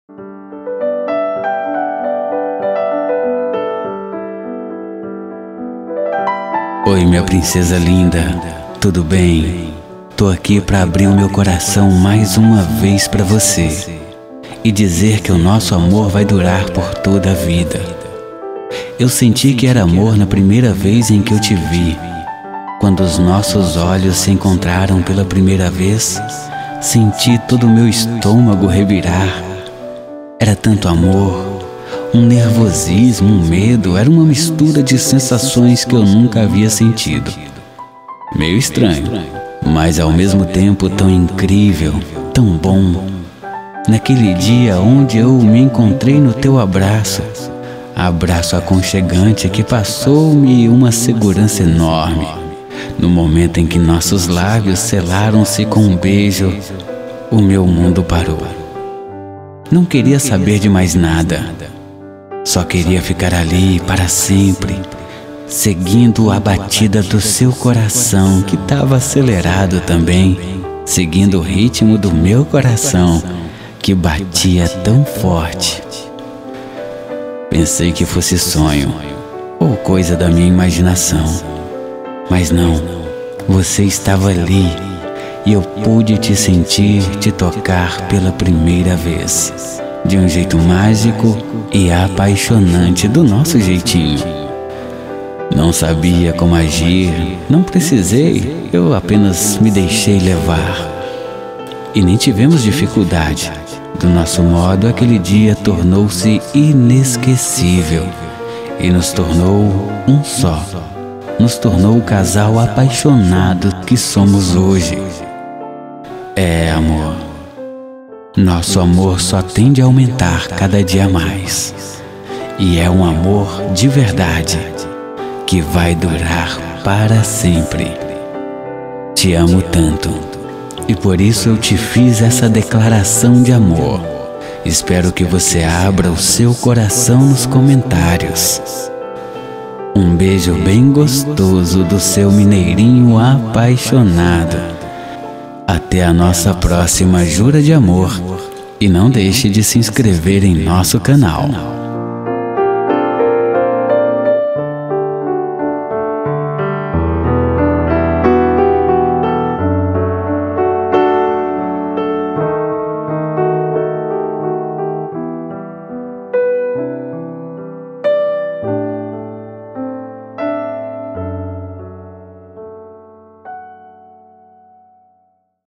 Te Amo Desde O Primeiro Dia 💕 💘
Encontre aqui as mensagens mais lindas para dizer *Eu Te Amo* de uma forma apaixonada e emocionante. Palavras românticas e cheias de doçura para compartilhar... 💌

Assista ainda nossa Playlist com as melhores e mais lindas Declarações de Amor e Mensagens de Carinho: